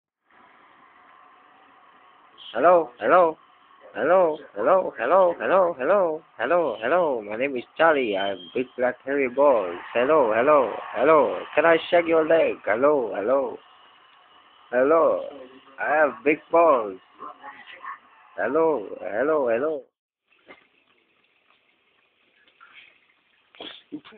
dog talking rude
this dog is fuking rude, and one horny boy,